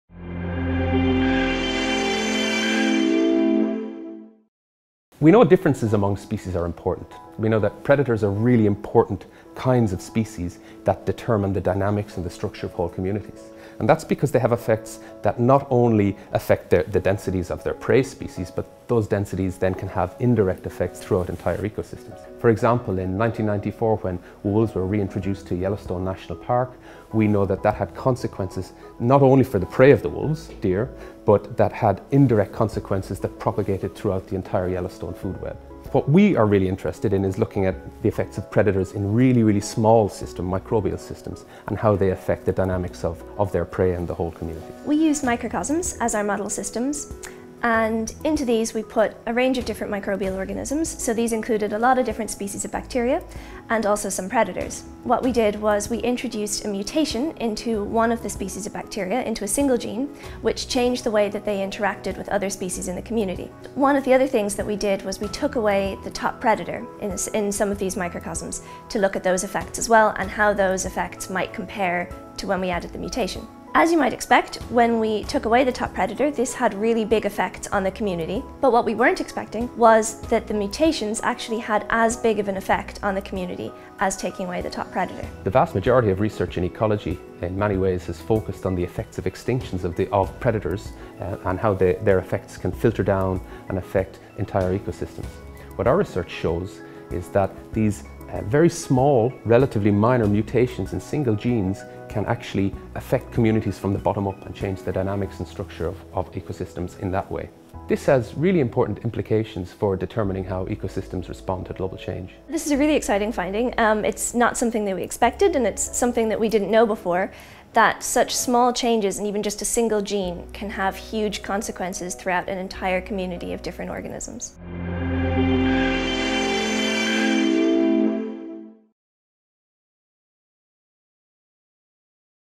Genetic Mutants Can Alter Entire Biological Communities
Scientists from Trinity College Dublin have discovered that one gene mutation in a single species can trigger dramatic change in whole biological communities – as much as that caused by the extinction of a top predator.

Speakers:
Assistant Professor in Zoology, School of Natural Sciences, Trinity College Dublin, Ian Donohue
PhD Researcher in Zoology, School of Natural Sciences, Trinity College Dublin, Deirdre McClean